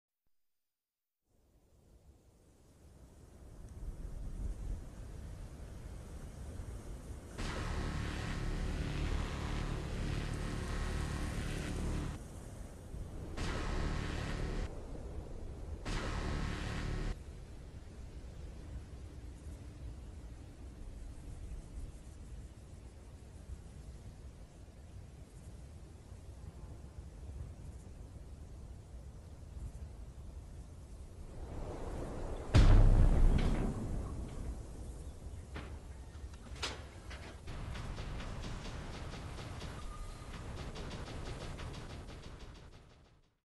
War Robots in real life || art imagery
sorry for re-uploads but this video has no music or commentry just pure art for those who didn't want to hear the story.